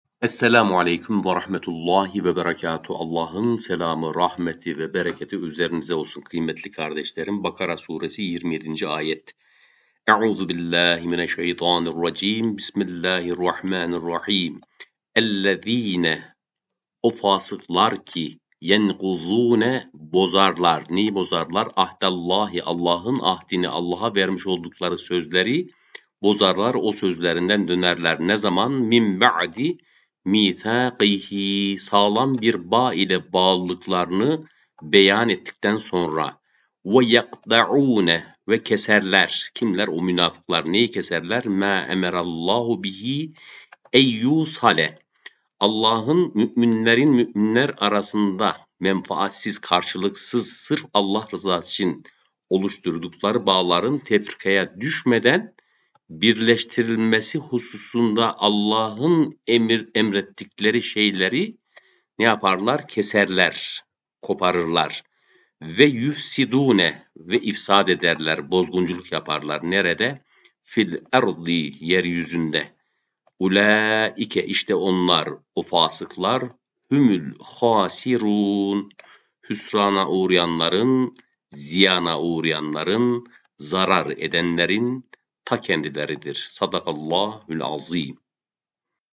BAKARA SURESİ MEALİ (27. AYET)
KUR'ÂN-I KERÎM MEALİ
Bakara suresi 27. ayet âyet kelime meali.
Çalışmamızda Elmalılı Hamdi Yazır (r.a.), Ömer Nasuhi Bilmen (r.a.), Diyanet ve Ahmet Varol'un meal ve tefsirlerinden istifade edilmektedir.
ALLAH hepsinden razı olsu